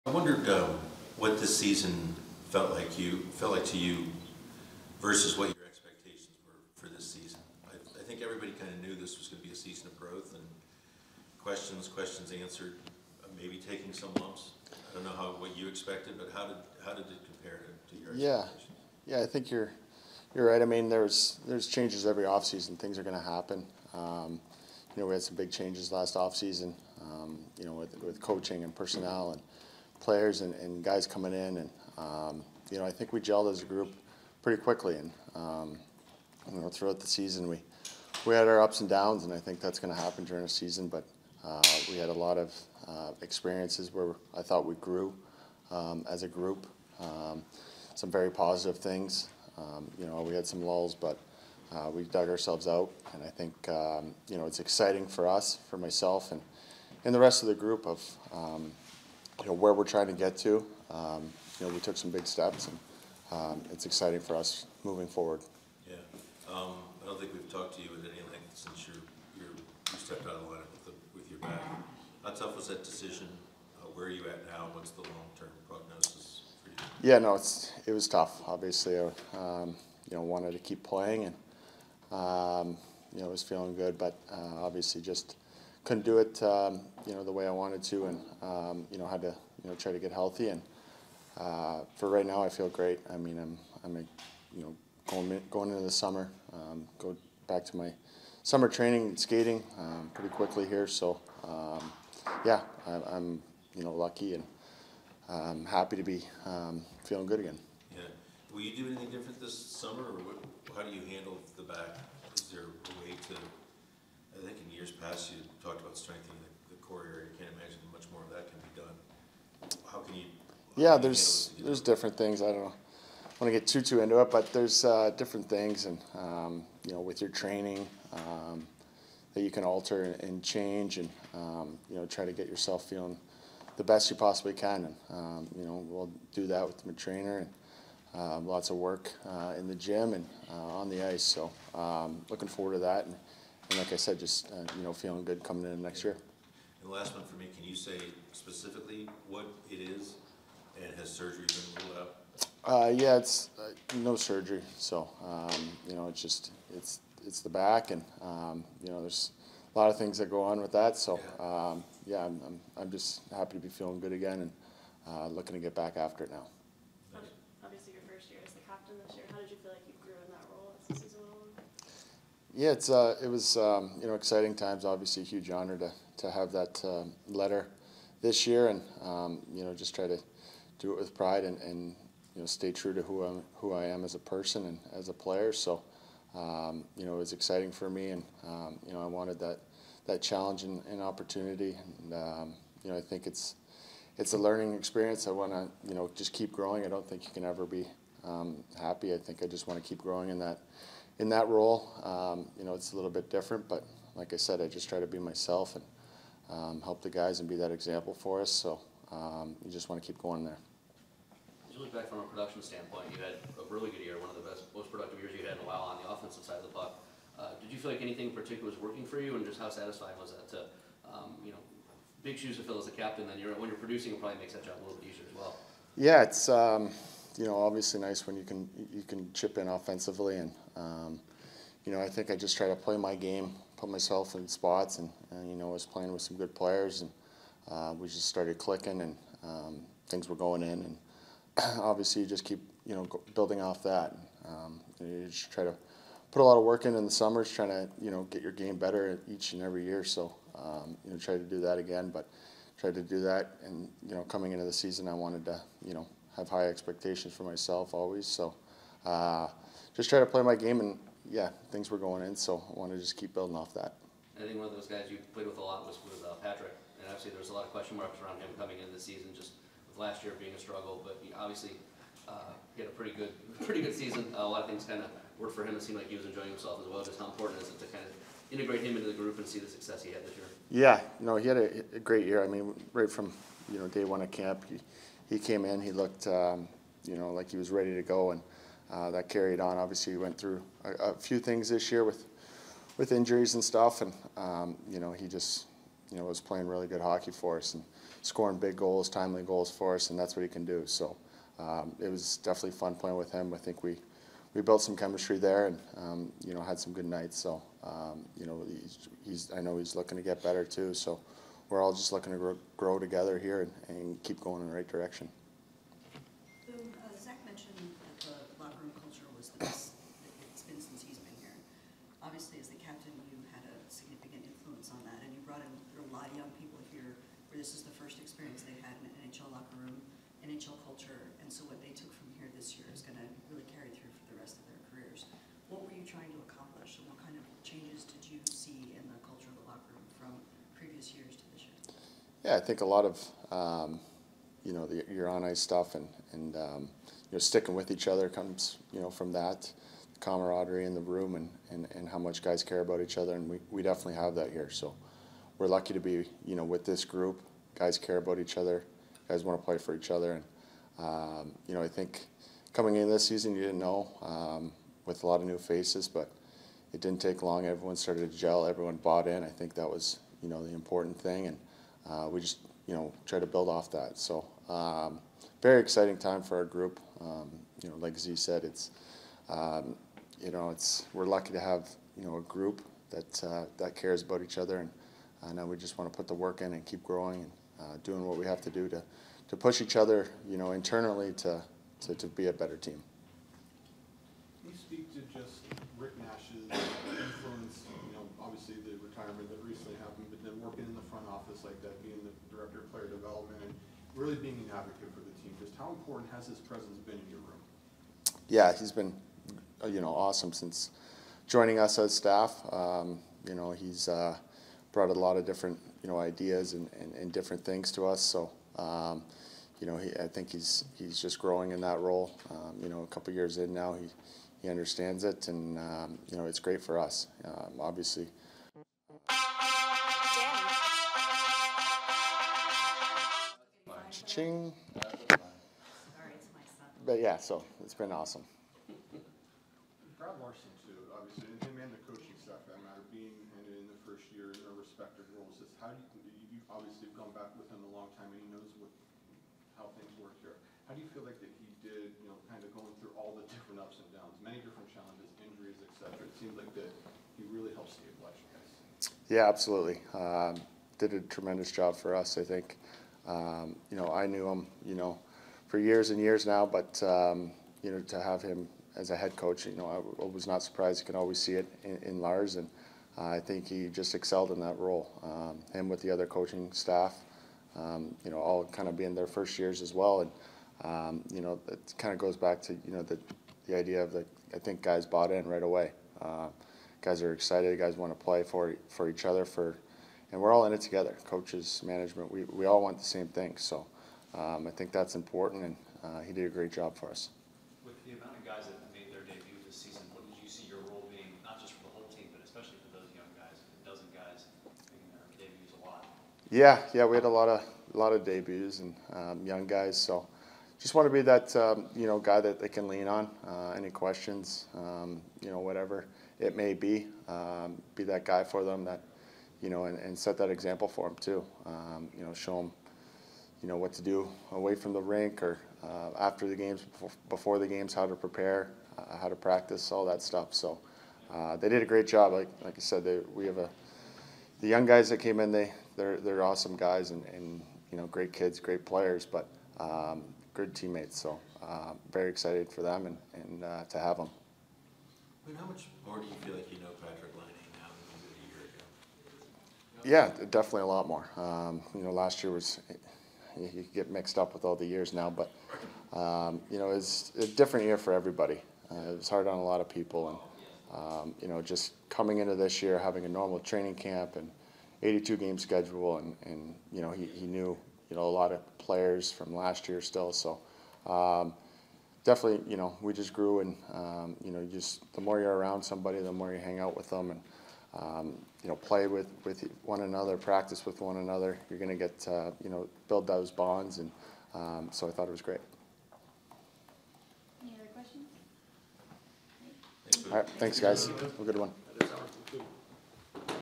Captain Boone Jenner says the CBJ took big steps this season
Boone Jenner speaks to the media at end-of-season exit interviews on April 30, 2022.